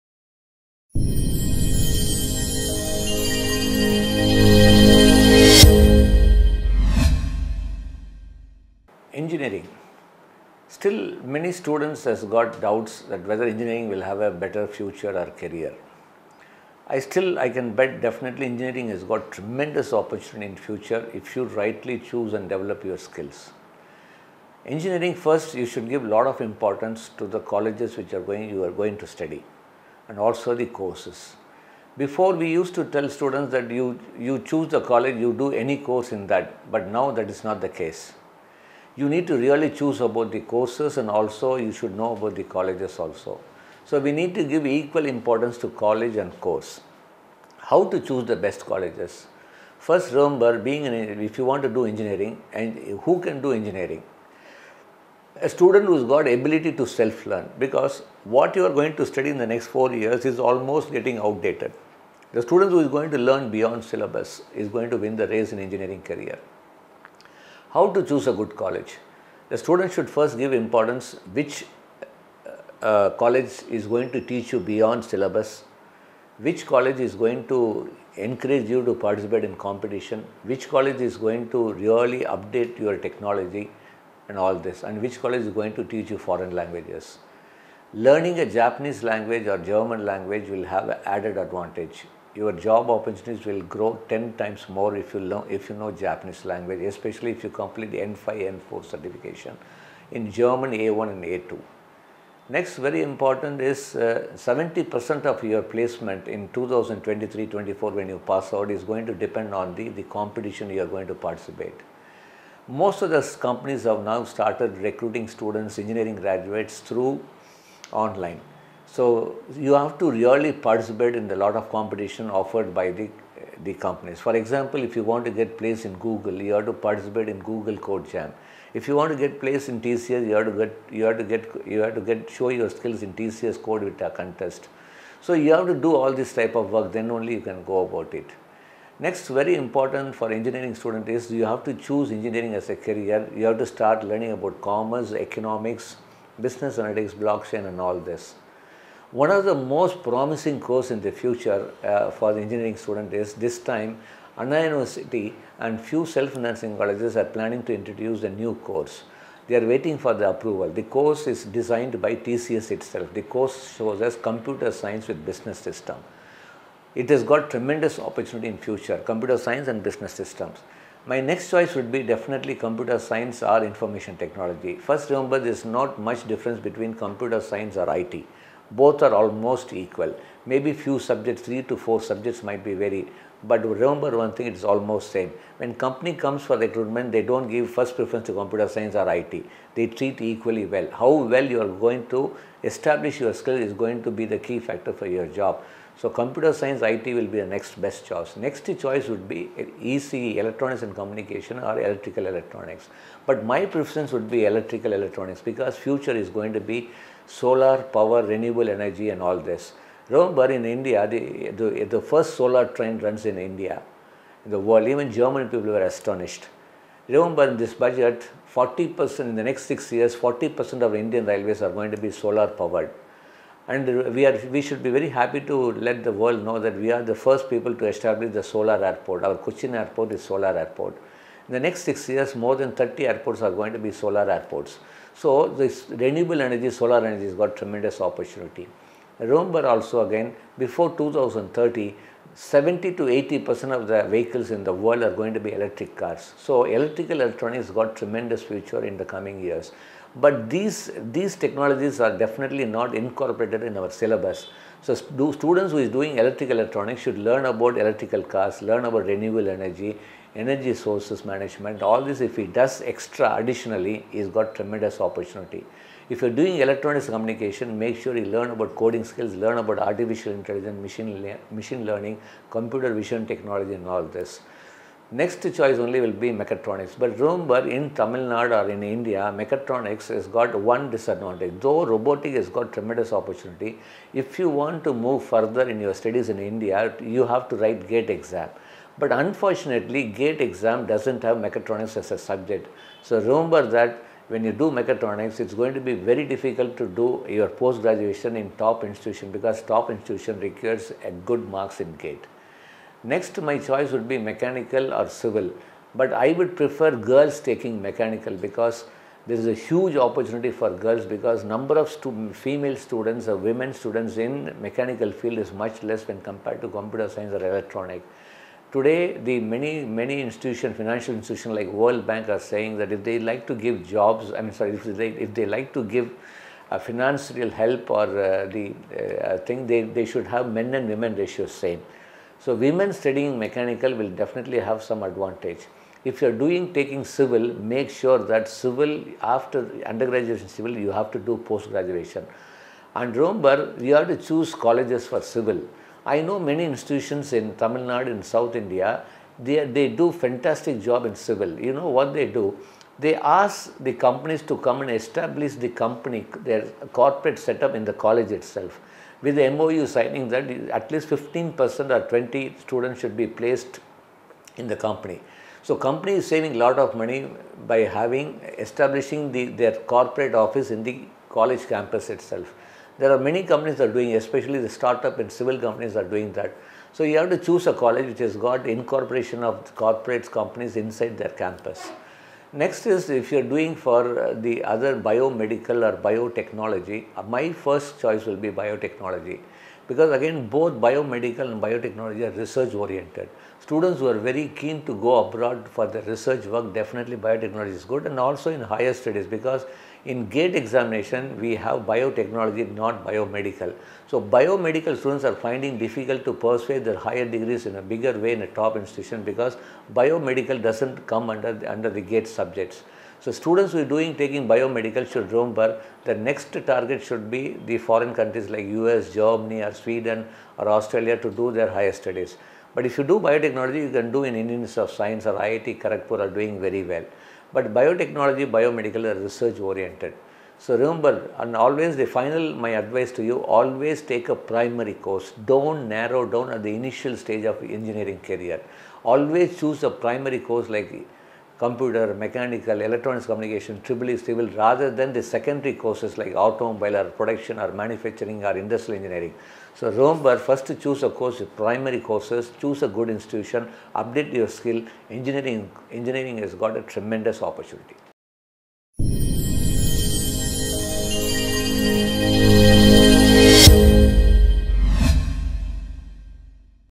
CAREER PLANNING AND OPPORTUNITIES FOR ENGINEERING GRADUATES |Jayaprakash Gandhi
General motivation program conducted through State Council of Educational Research and Training - TamilNadu for students on how to shape their future by Jayaprakash Gandhi - Career Guidance and analyst who has been in the industry for over 18 years and has counseled over 1.5 million students.
Career Guidance,
Shreeji Complex, IInd Floor,
New Bus Stand West, Salem,
TamilNadu,

Phone:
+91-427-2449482